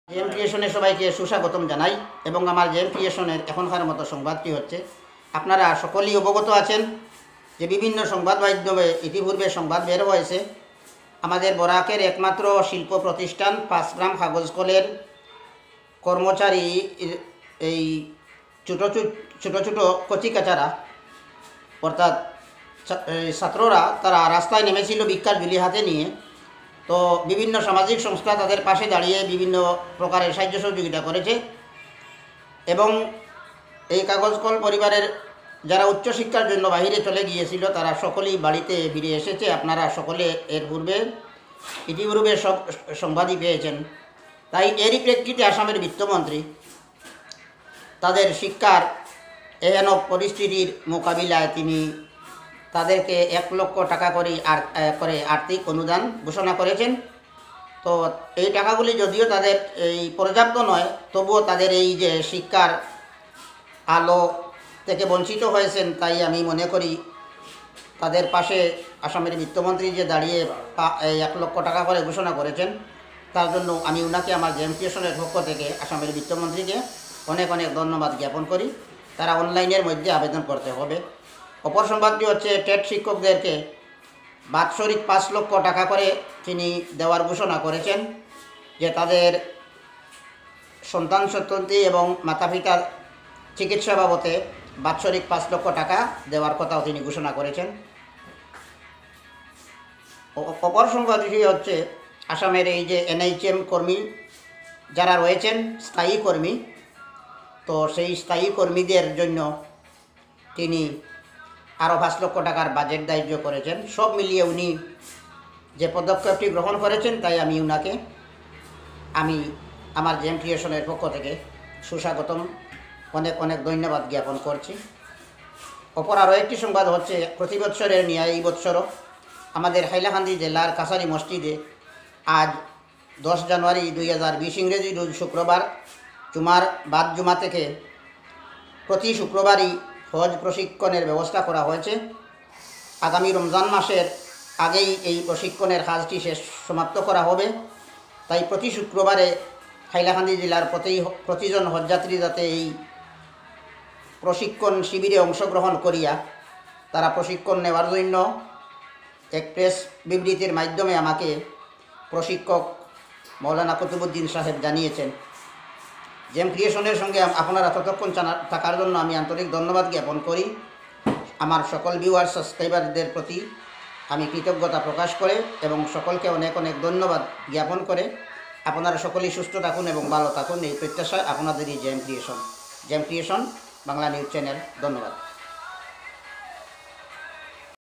আসামের বিত্ত মন্ত্রী বেশ কয়েকটি প্রকল্প হাতে নিয়েছেন।
আসামের বিত্ত মন্ত্রী বেশ কয়েকটি প্রকল্প হাতে নিয়েছেন। উল্লেখযোগ্য কাগজ কল পরিবারের জন্য আর্থিক সাহায্য প্রদান।